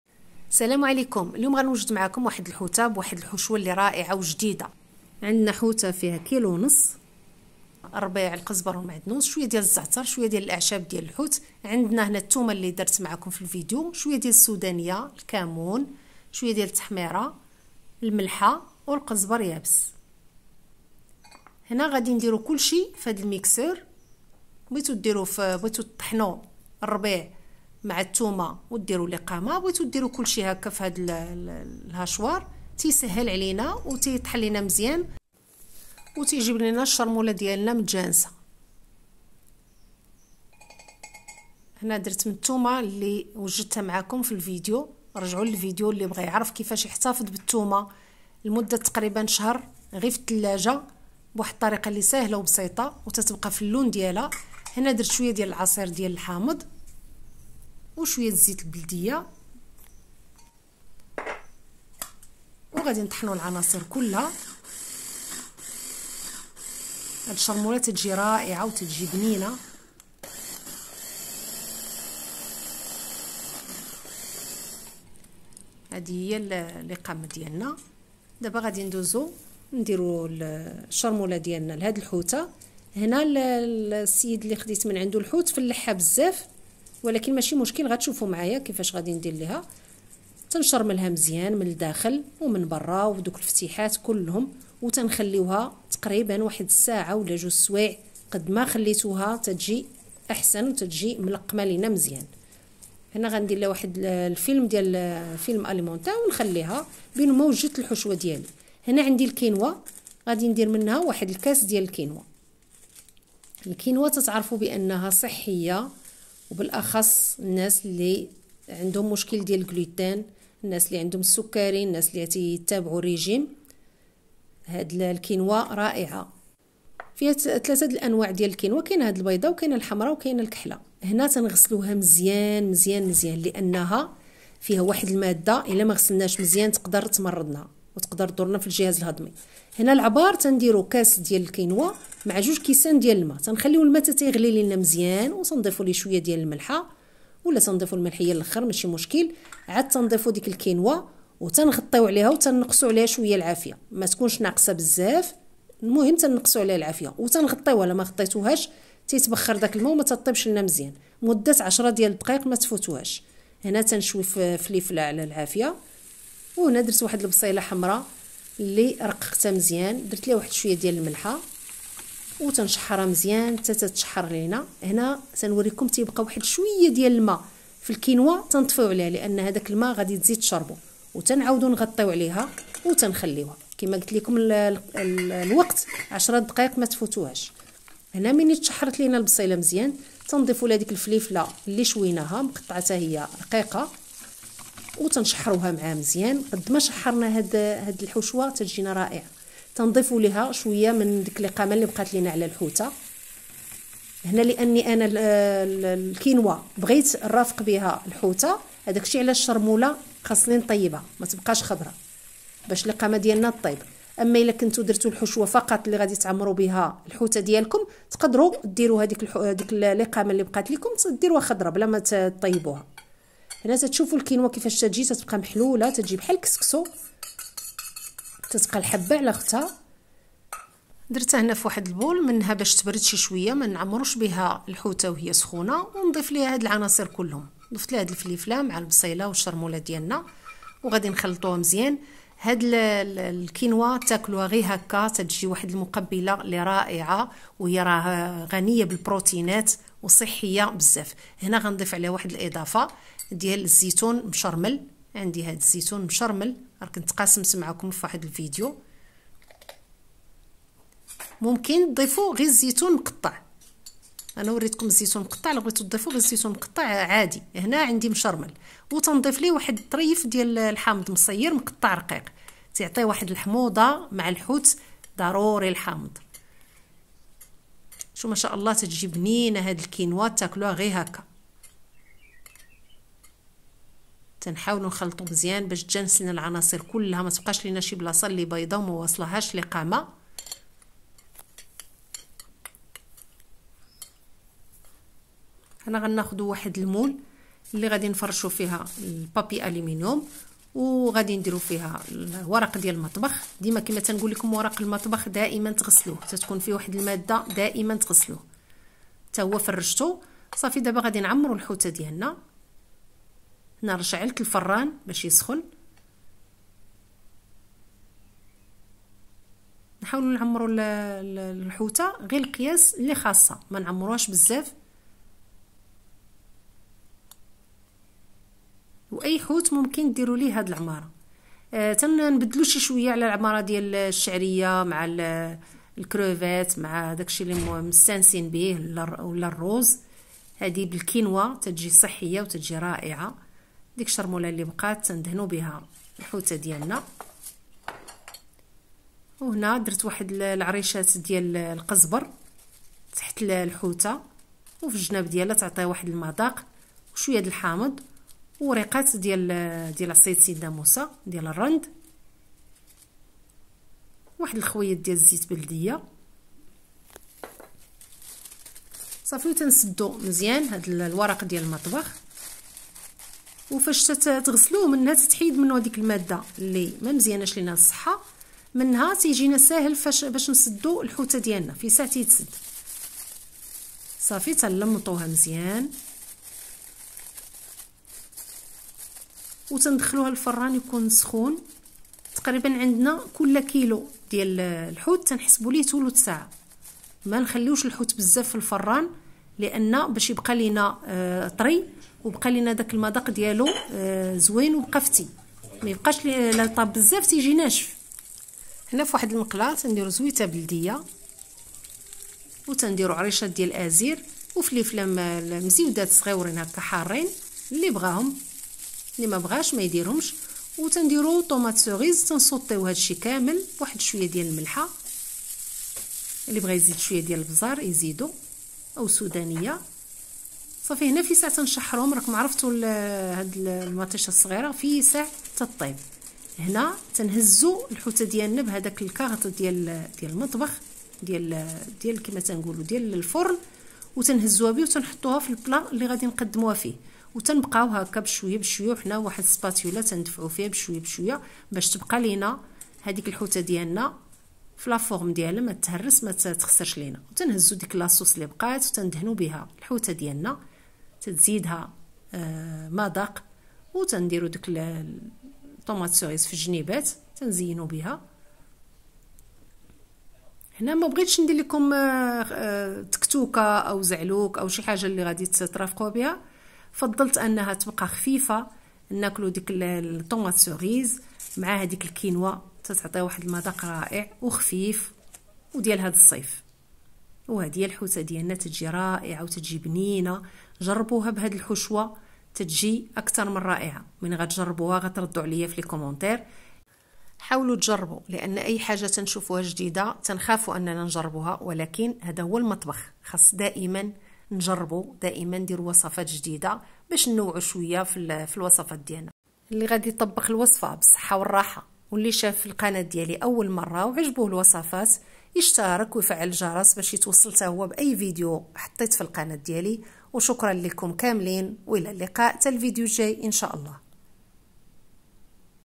عمرك كليتي الحوت معمر بهاد الحشوة تدجي واعرة حشوة جديدة ومبتكرة الى جربتيها غادي تعجبك ان شاء الله
حوتة_معمرة#وصفات_سهلة